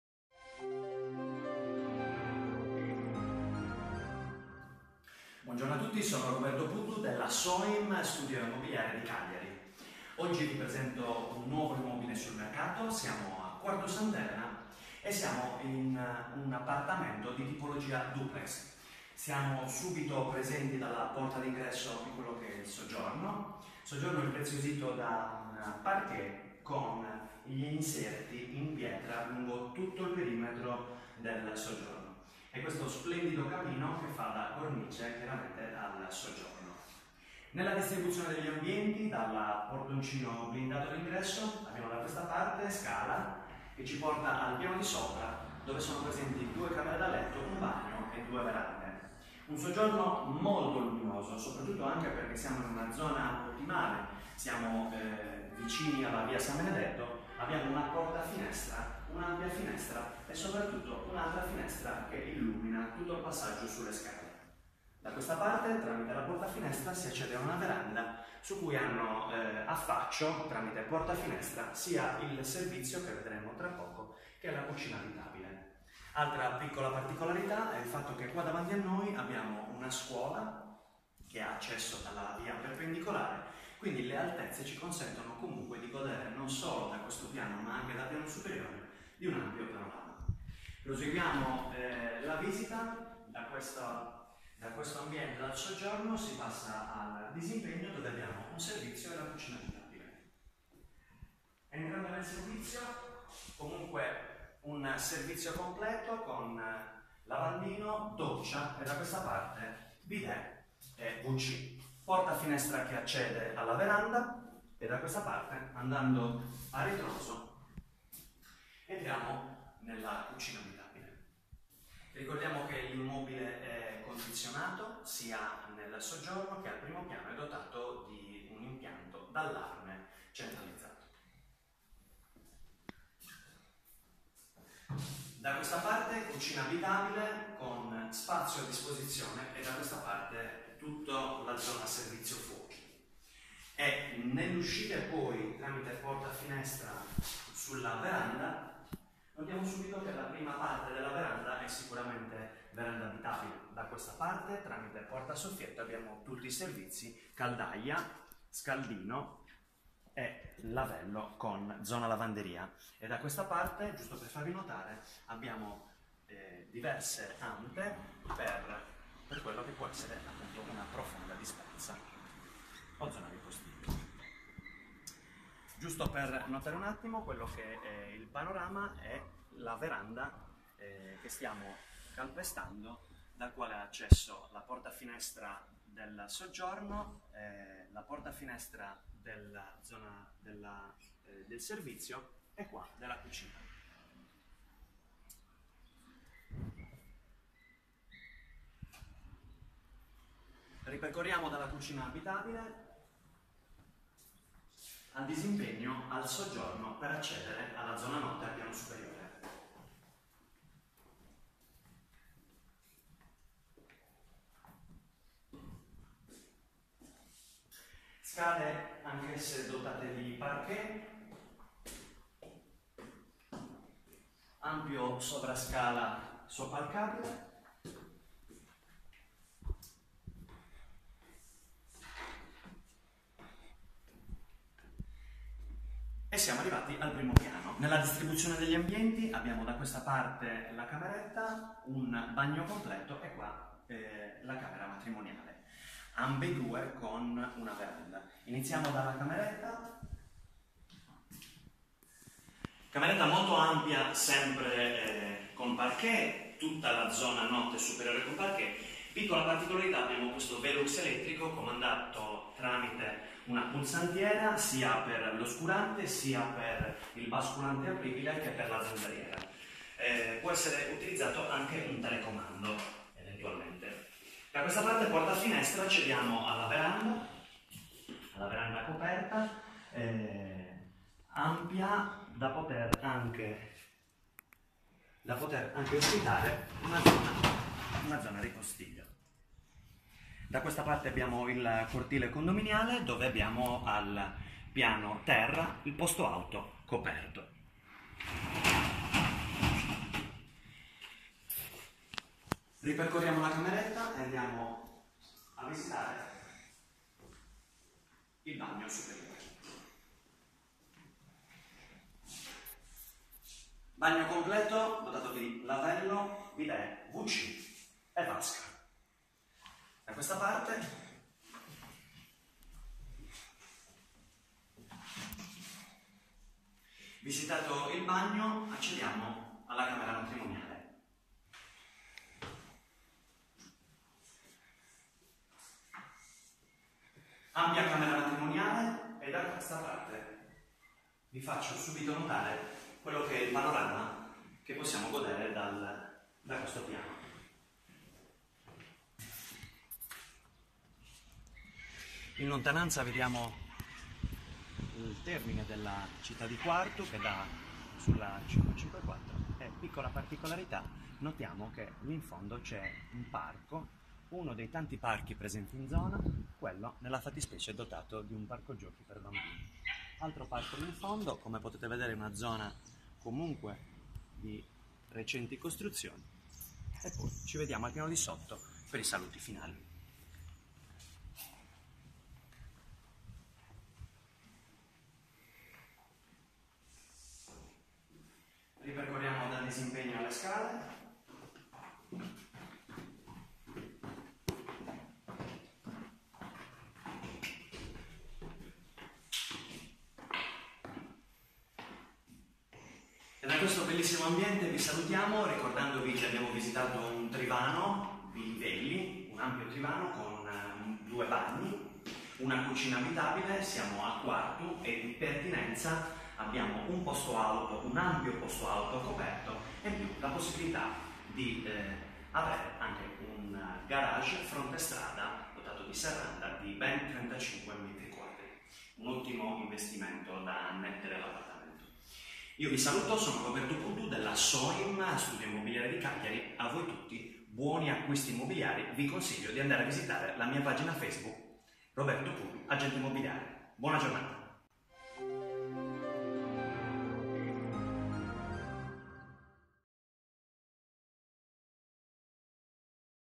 QUARTU - Trivano ultimo piano Bilivelli con p.auto coperto (ROBERTO PUDDU Agente Immobiliare Soimm)
Descriz. Immob.: A Quartu in una strada parallela a alla via San Benedetto, all'incrocio tra la via Ariosto e la via Cannelles, presentiamo un Particolare immobile duplex su due livelli all'ultimo e penultimo piano.
La tipologia è trivano, ampio per dimensioni e distribuzione di ambienti.
L'immobile apre sul soggiorno, impreziosito dal parquet e da rifiniture in pietra e da un ampio camino; l'ambiente è ben illuminato grazie alle 3 aperture tra finestre e portafinestre che conducono alla veranda.
Dal disimpegno si accede al servizio completo con doccia e alla cucina abitabile che si affaccia alla veranda abitabile con zona lavanderia e ripostiglio.
Dal soggiorno tramite una scala rivestita in parquet si accede all'ultimo piano superiore mansardato dove troviamo: una camera con veranda e ripostiglio esterno e ulteriore finestra velux motorizzata a soffitto; un bagno completo con vasca; un'ampia camera matrimoniale con balcone dal quale si gode una vista panoramica.
L'immobile si presenta in buone condizioni, ed oltretutto ha nel cortile condominiale un ampio posto auto coperto.
Abbiamo altressì la possibilità di valutare al piano terra fronte strada un box auto di circa 30 mq dotato di servizi.
Un dato interessante oltre la zona ben servita il fatto che l'edificio fronte immobile è ben distante ed è una scuola che arriva solo al primo piano consentendo oltre che privacy anche di avere un buon panorama.
Rif: RP105.
Cordialmente Vi saluto.
Buona giornata a tutti.